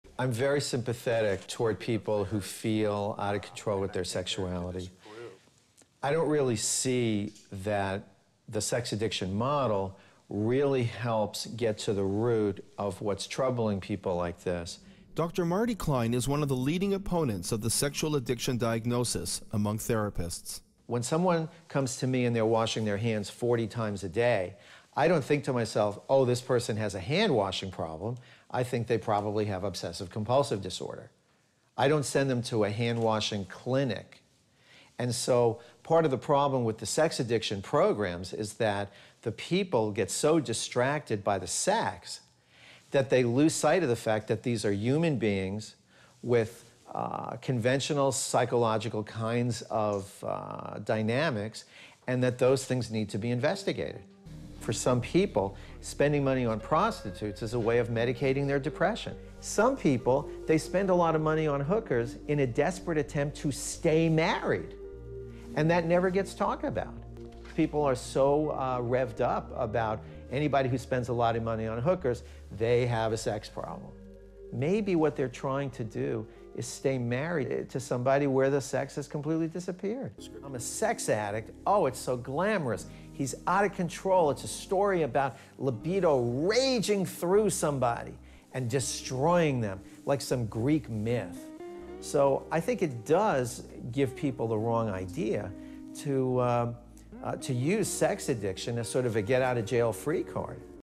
Dr. Marty Klein--Canadian TV Corp.
"Sex addiction"

"I'm very sympathetic toward people who feel out of control sexually. But the sex addiction model doesn't address what's really troubling these people. People who wash their hands 40 times a day don't have a hand-washing problem, and don't need a hand-washing clinic. Sex addiction programs are so focused on sex, they forget that these are human beings with psychological & relationship dynamics, which must be investigated. Going to hookers can be a way someone medicates their depression, or it might be a desperate attempt to stay in a sexless marriage."